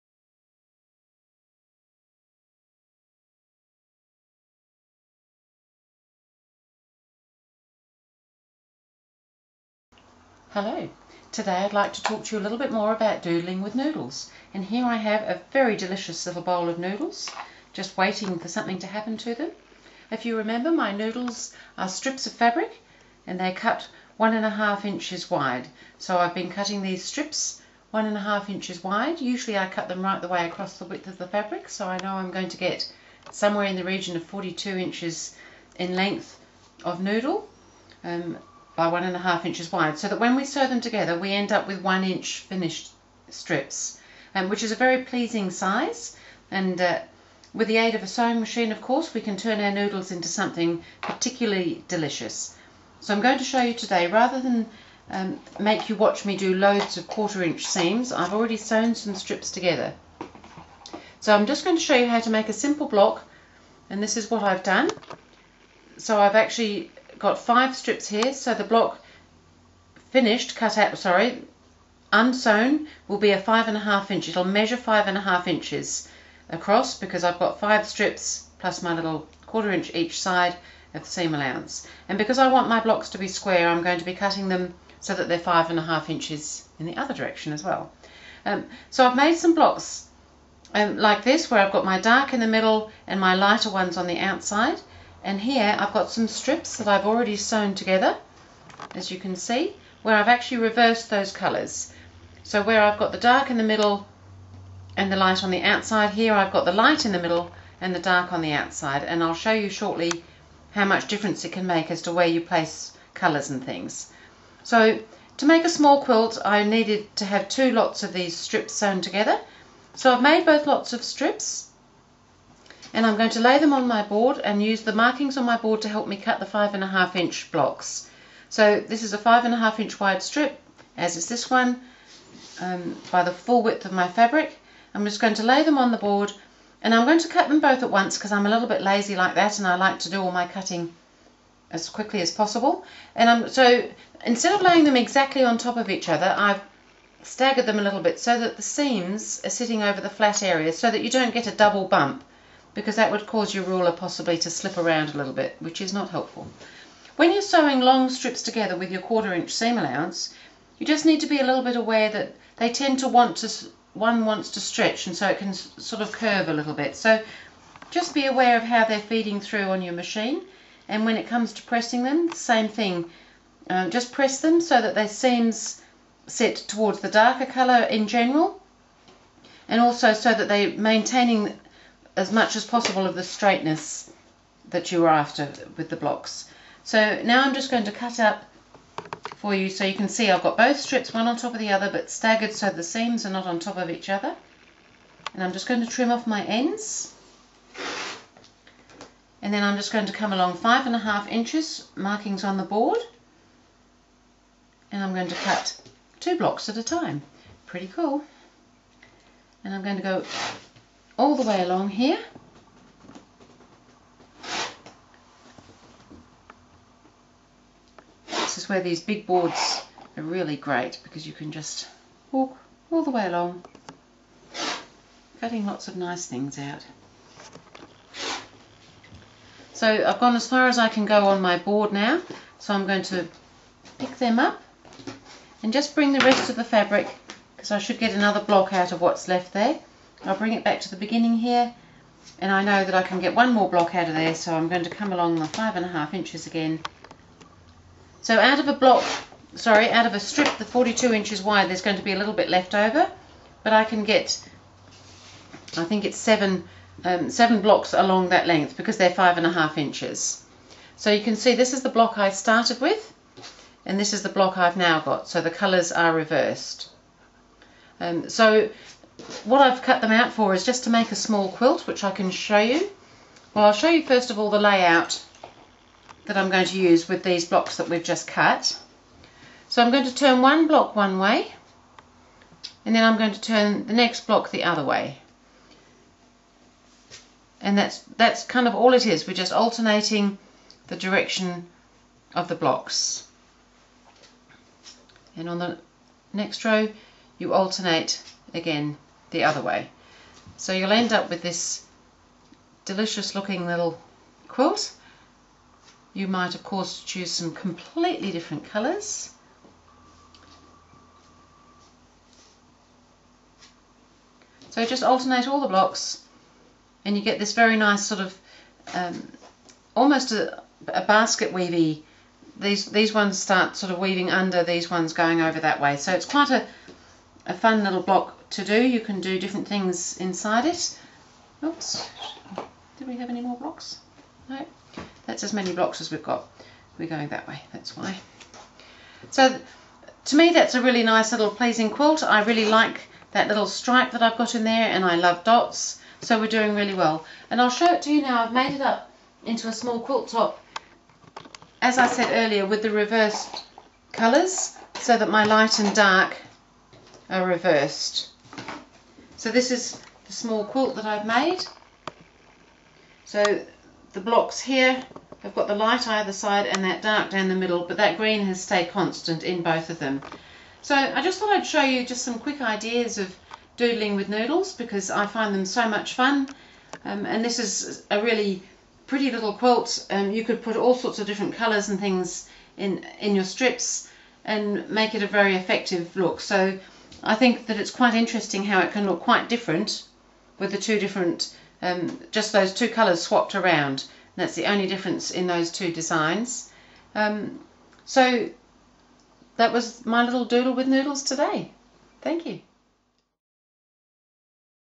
Doodles with Noodles 2 - or playing with 1 1/2" strips - Quilting Tips & Techniques 053
Don't you just love noodles? Here I will show a simple idea that will make your noodles quite delicious!